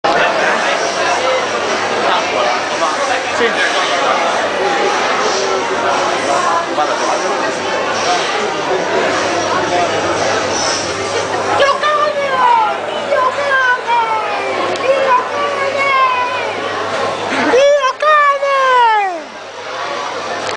Alunni vs Hitler
Museo delle Cere alias Madame Tussauds...desideravo farlo da troppo tempo!